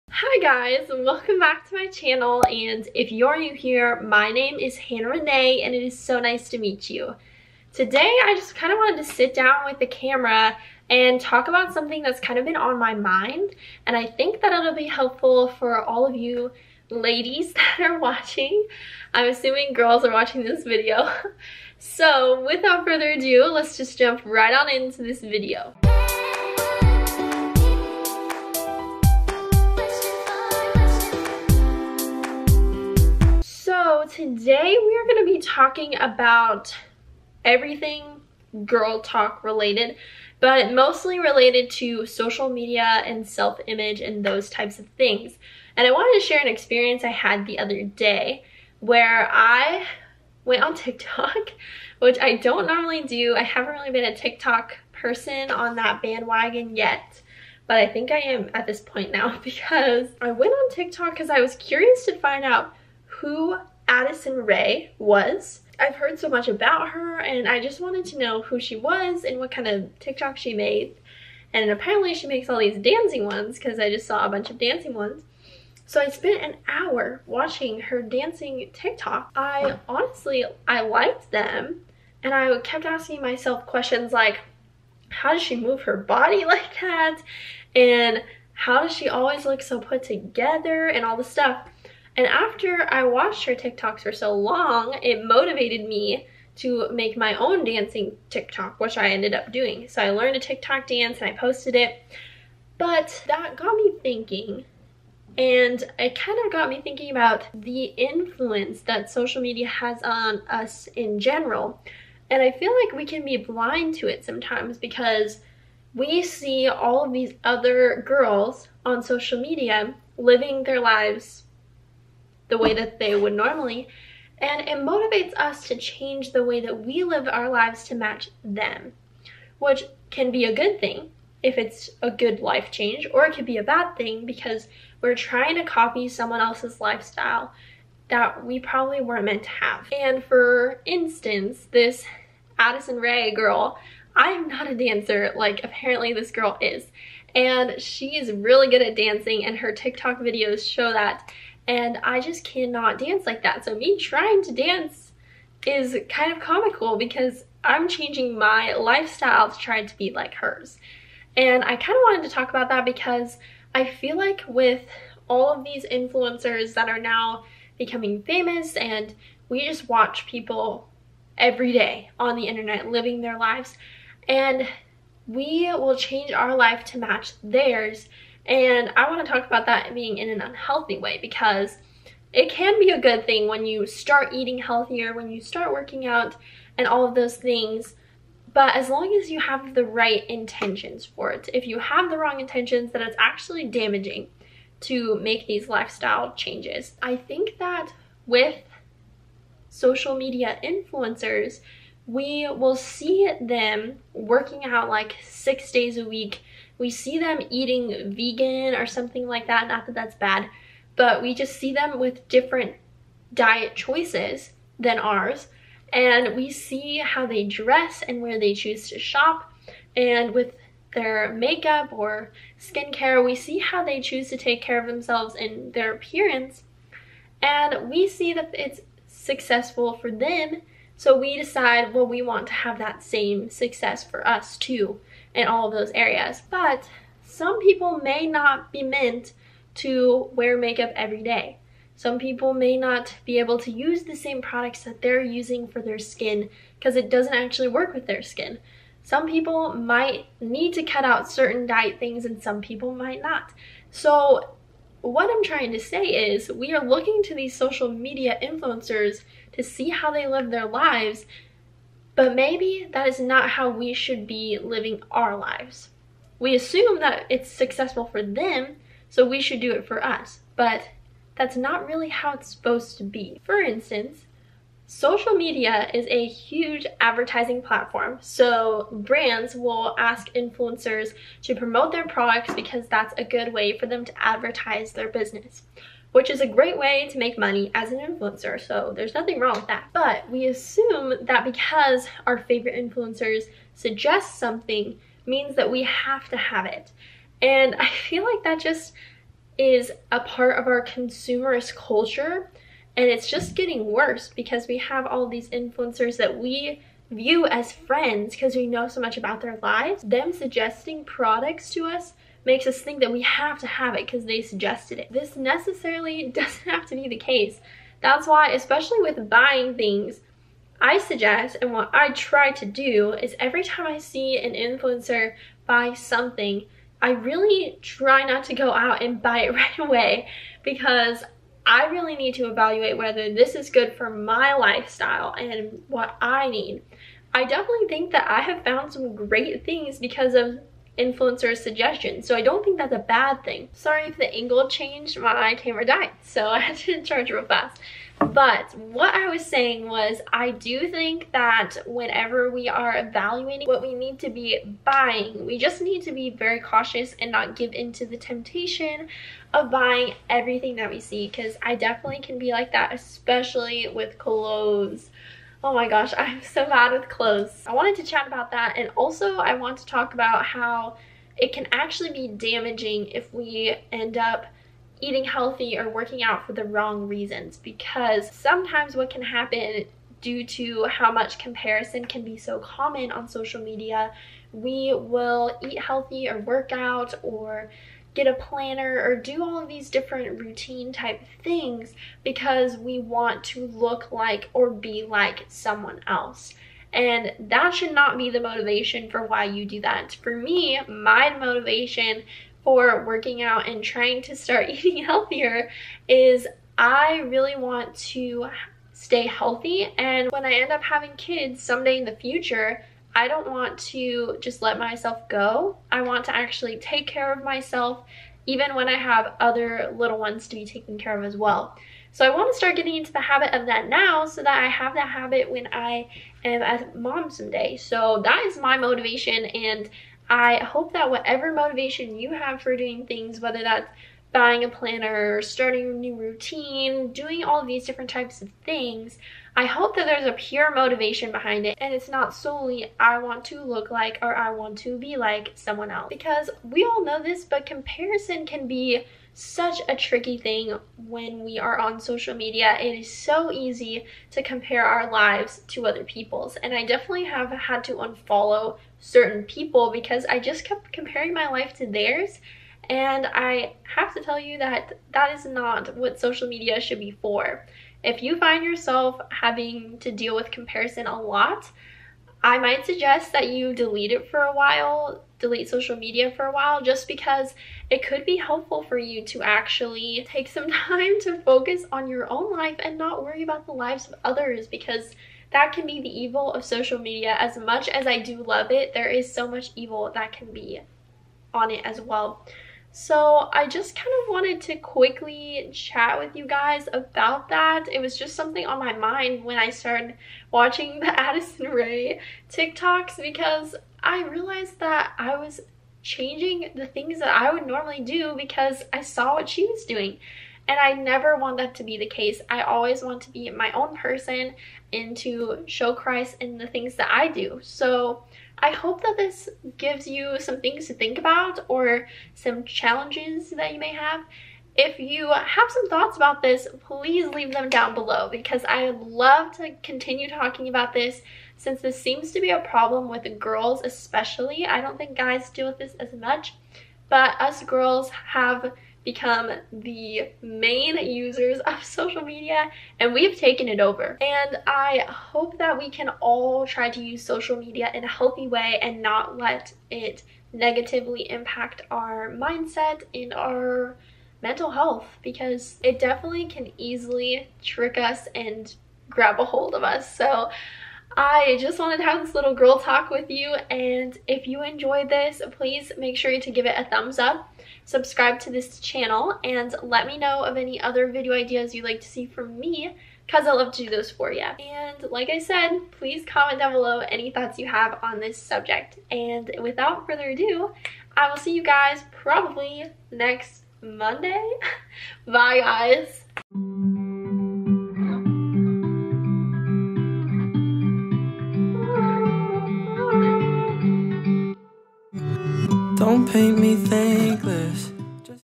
GIRL TALK//let's talk comparison, self image, and social media influencers
I wanted to talk about something has been on my mind lately as I have been following social media influencers for the last few years and watched them have a huge impact on my personal life and the personal lives of other social media loving females. Let me know if you have any thoughts about this topic below!

Instagram & Tik Tok: hannahreneebanana
Poshmark: hannahrenee1319
VSCO: Hannah Renee